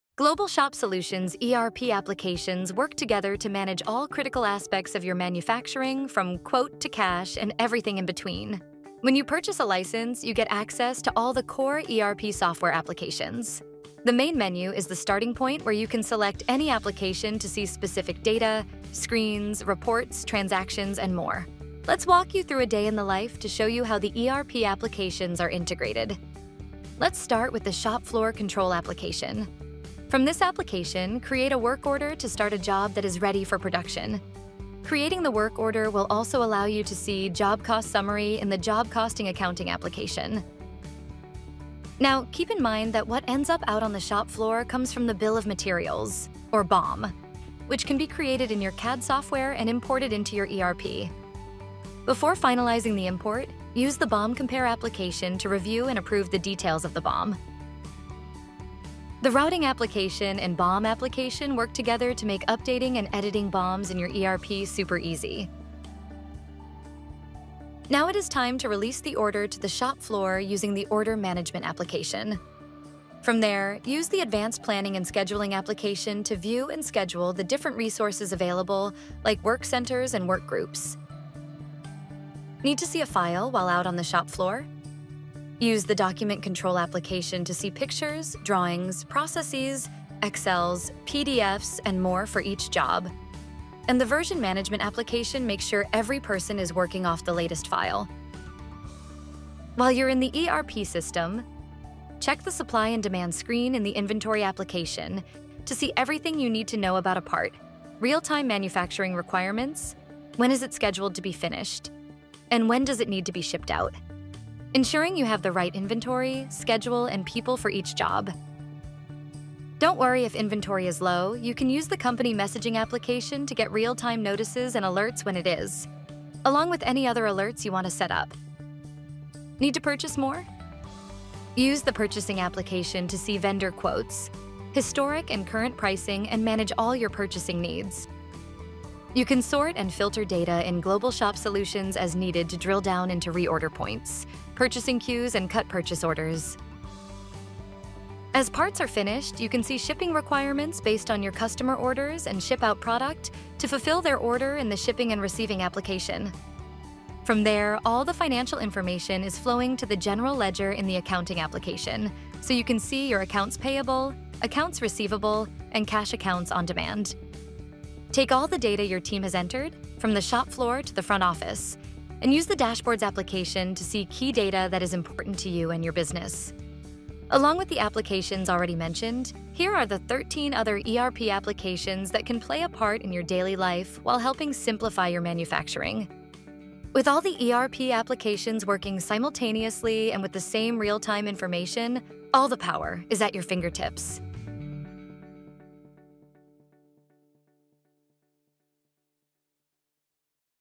ERP Applications in 3 Minutes
Dive into the ERP applications that can simplify your manufacturing from quote to cash with Global Shop Solutions ERP software. See the applications in action with a product tour